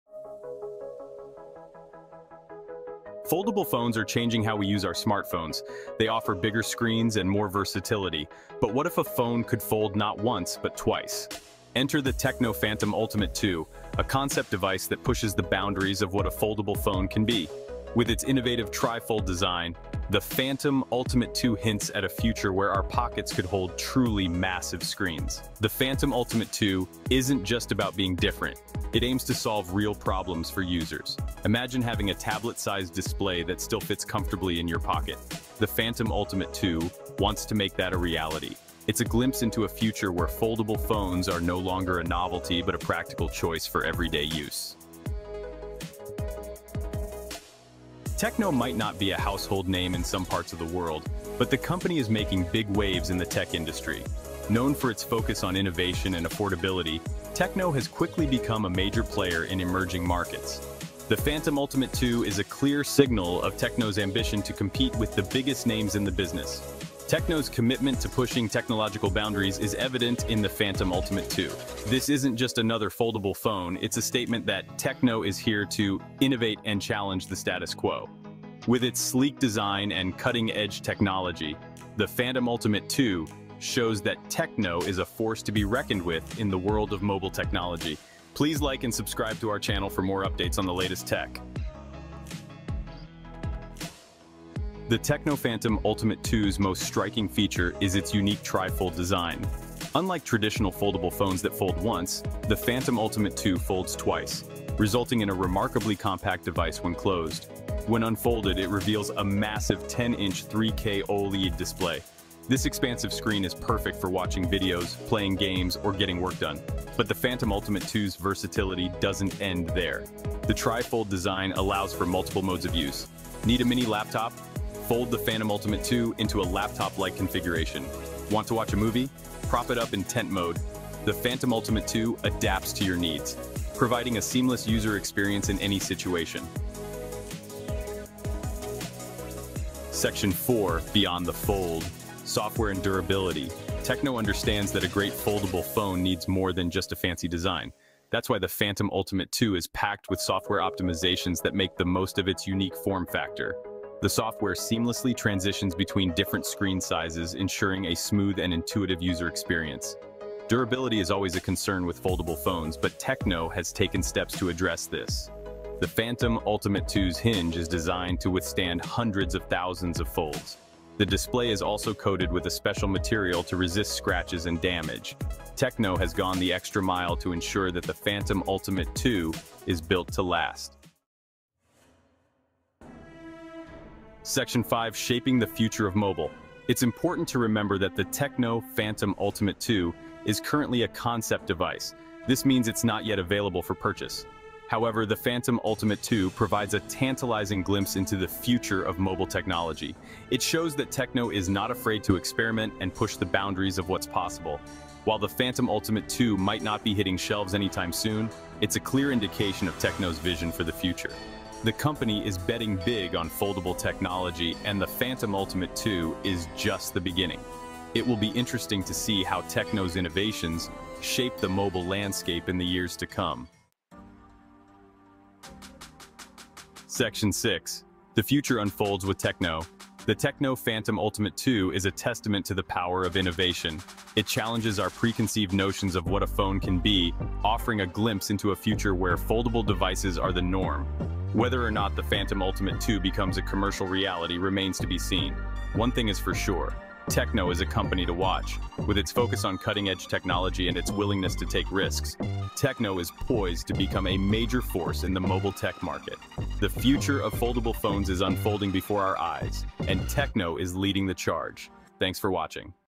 TECNO Phantom Ultimate 2: The Revolutionary Tri-Fold Phone That’s Redefining Foldables
Get ready to witness the future of smartphones with the TECNO Phantom Ultimate 2! In this video, we dive deep into TECNO's groundbreaking tri-fold phone, showcasing its innovative design, versatile functionality, and unmatched durability. With a sleek 11mm thickness and a stunning 10-inch OLED display, this concept device is setting new standards in mobile technology. Is this the foldable you've been waiting for?

OUTLINE:

00:00:00 A New Era of Foldable Phones
00:00:44 An Emerging Tech Giant
00:01:31 Unveiling the TECNO Phantom Ultimate
2
00:02:16 Software and Durability
00:03:01 Shaping the Future of Mobile
00:03:43 The Future Unfolds with TECNO